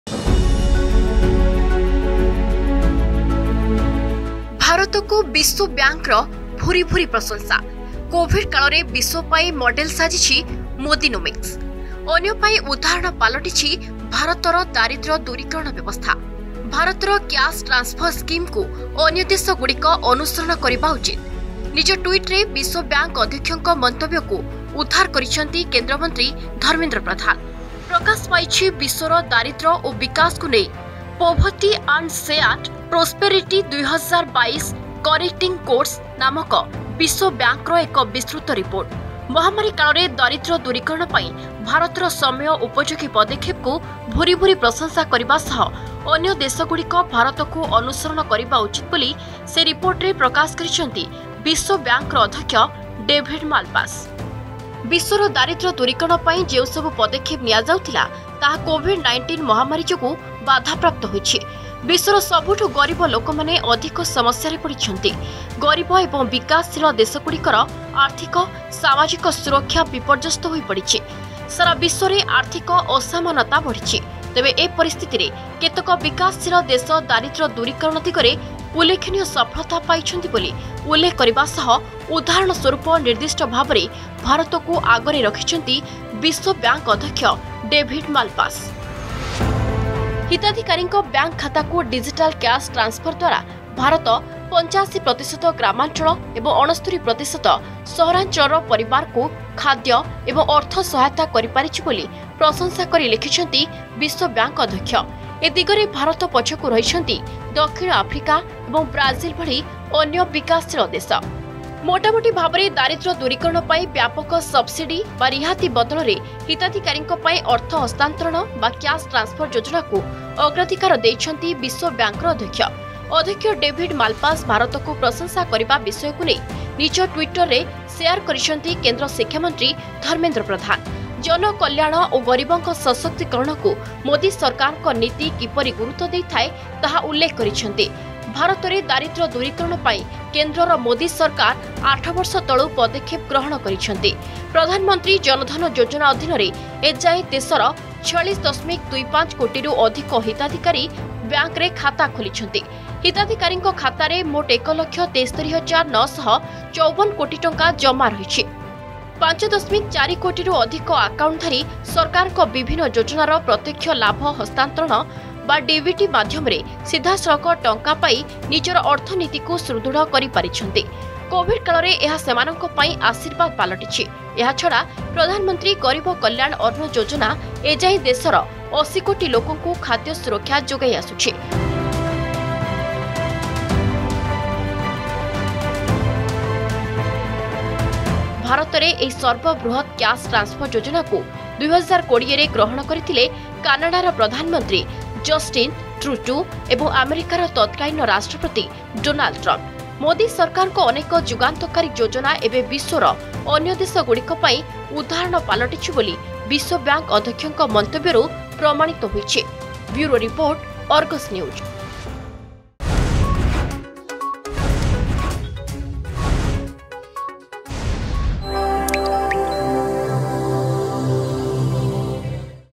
ଭାରତକୁ ବିଶ୍ବବ୍ୟାଙ୍କର ପ୍ରଶଂସା
ଭାରତଙ୍କୁ ବିଶ୍ୱବ୍ୟାଙ୍କର ଭୁରି ଭୁରି ପ୍ରଶଂସା କୋଭିଡ୍ କାଳରେ ବିଶ୍ବ ପାଇଁ ମଡ଼େଲ ସାଜିଛି ମୋଦିନୋମିକ୍‌ସ ଅନ୍ୟ ପାଇଁ ଉଦାହରଣ ପାଲଟିଛି ଭାରତର ଦାରିଦ୍ର୍ୟ ଦୂରୀକରଣ ବ୍ୟବସ୍ଥା